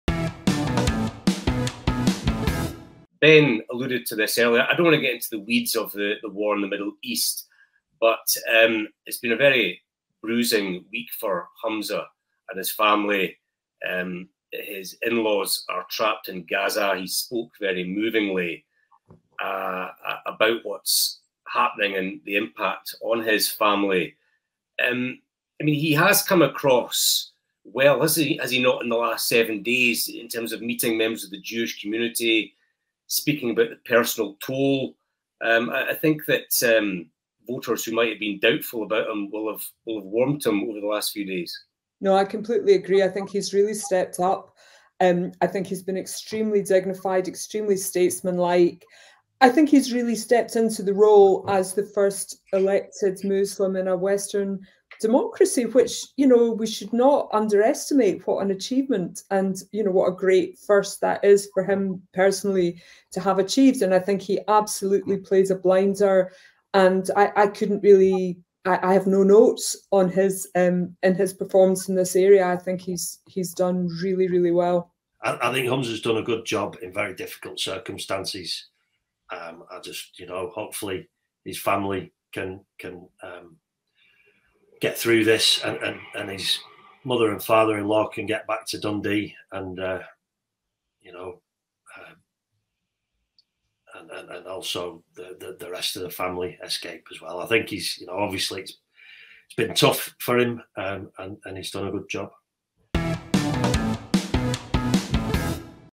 Humza Yousaf's Gaza response analysed | Planet Holyrood
First Minister Humza Yousaf’s in-laws are “trapped” in the Gaza war zone, with Nadia Maged El-Nakla’s parents visiting relatives in territory which Israel’s Prime Minister has promised will be reduced to “rubble”.

SNP MP Chris Law said Elizabeth El-Nakla had made her “final goodbyes” in a call with the First Minister and her daughter.

Paul Hutcheon, Anna Burnside and Ben Borland discuss the closing of the SNP's conference, their surprise council tax freeze, an unsurprising appearance by Nicola Sturgeon and Humza Yousaf's very personal response to the growing crisis in Gaza.